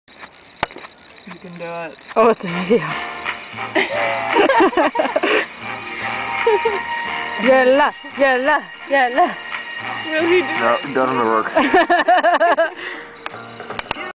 Smoking in the woods doesn't work
Jelle tried to used my "Light My Fire" flint stick to light his cigarette.  Perhaps he should just quit.